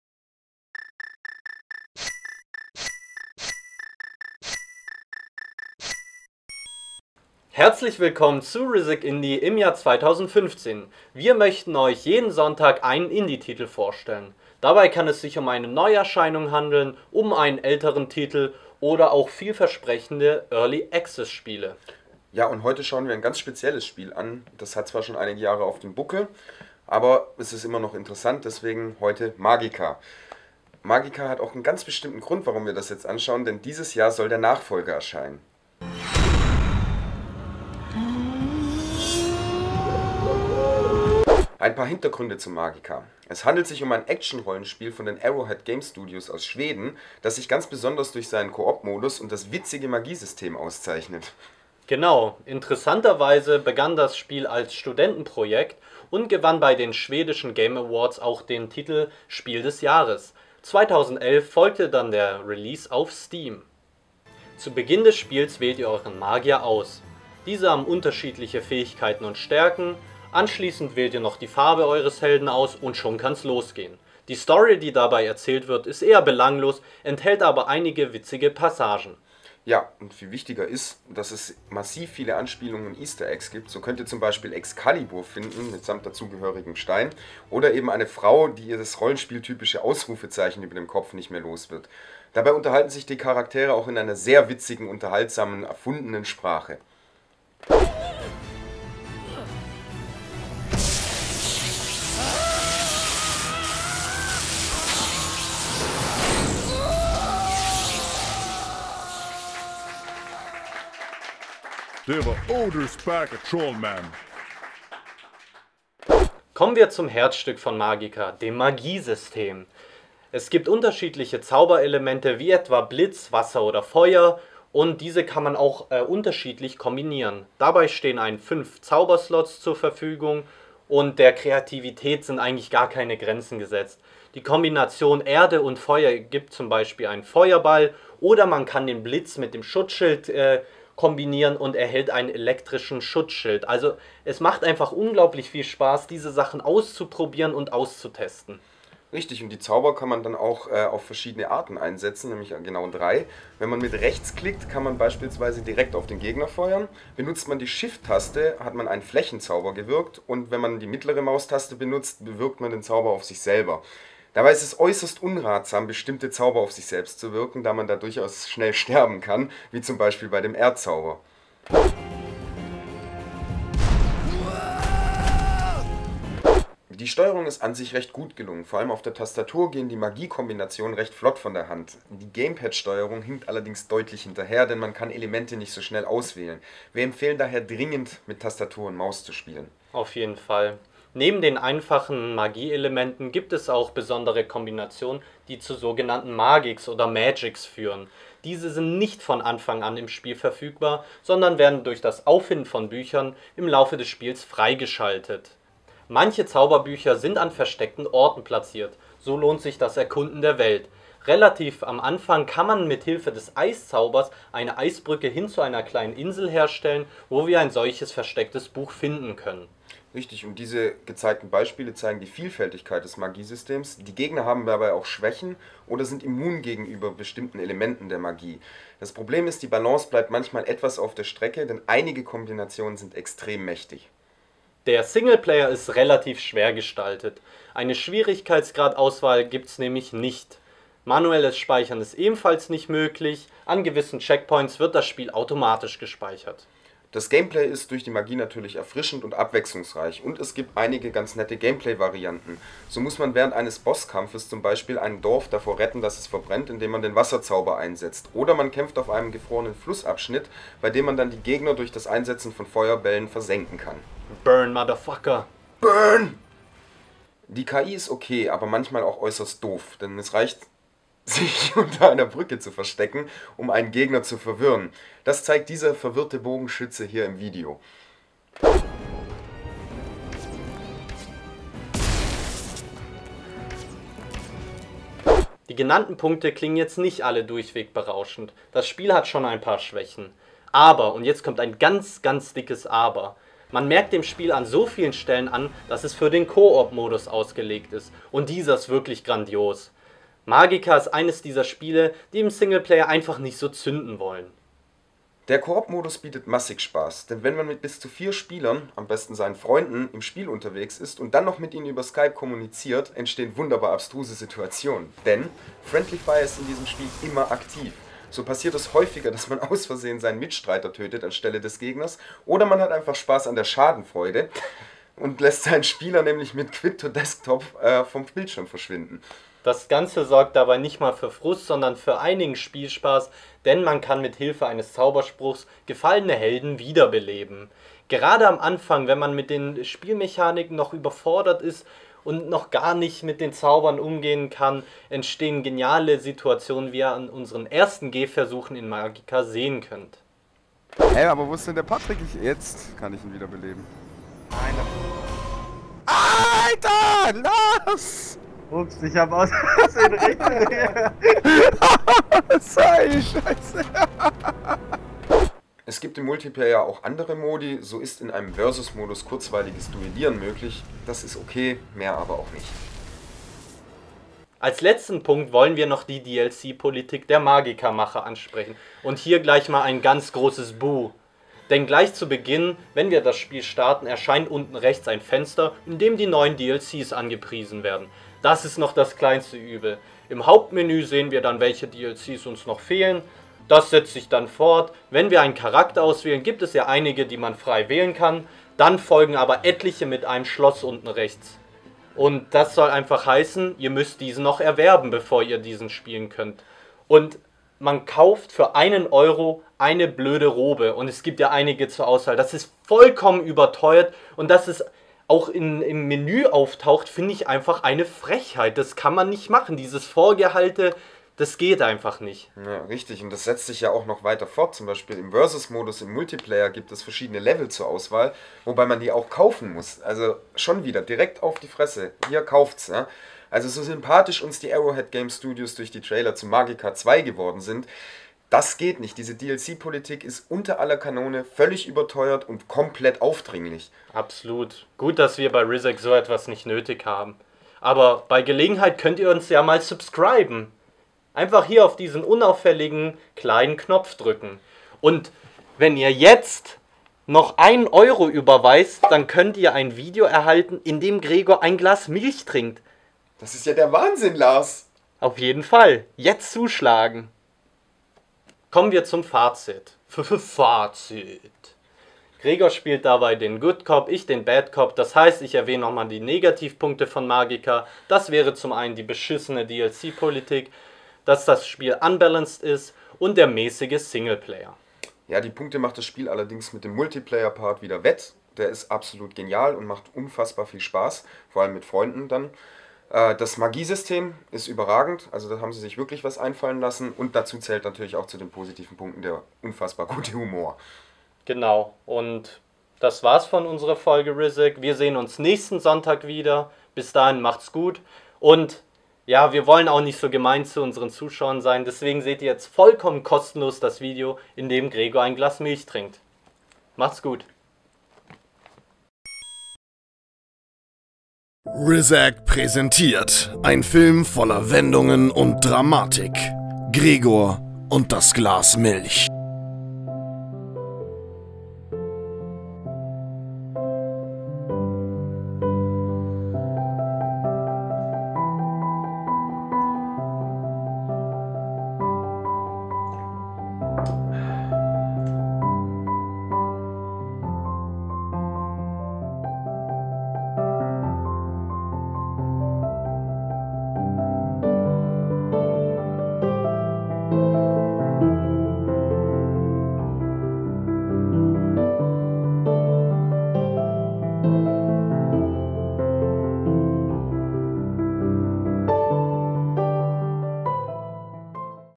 Magicka - Test / Review: Co-op Spaß mit sadistischen Zügen
RISAG Indie ist zurück. Und ab sofort im wöchentlichen Format! Jeden Sonntag wird ein Blick auf einen Indie Titel geworfen. Neue oder ältere Spiele sind dabei, genauso wie Early Access Titel. In der heutigen Folge geht es um Magicka.

Welche Indie Titel interessieren euch? Bitte schreibt dies in die Kommentare.